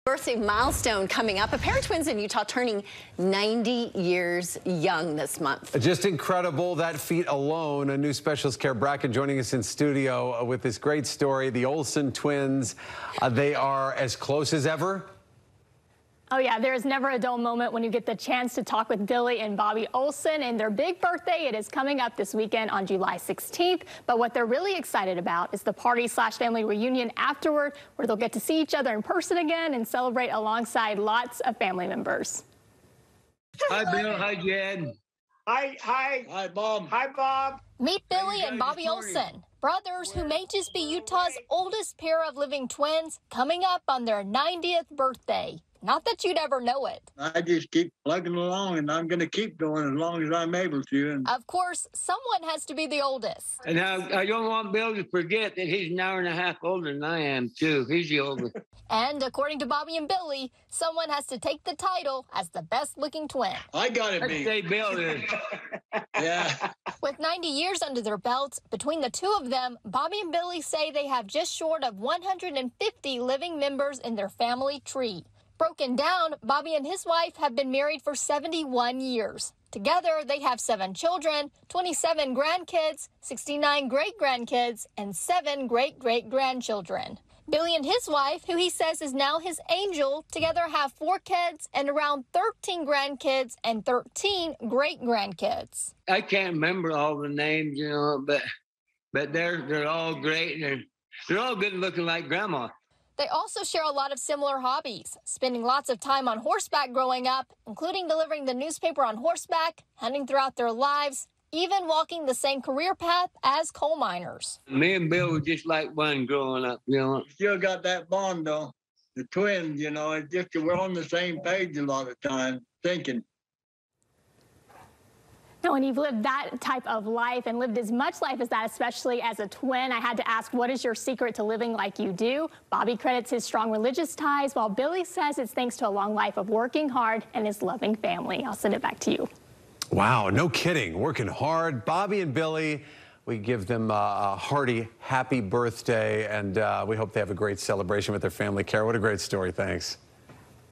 Utah twins to celebrate 90th birthday this weekend
A pair of Utah twins are coming up on a big milestone celebration. On Saturday, Bobbie and Billie Olsen will turn 90 years old.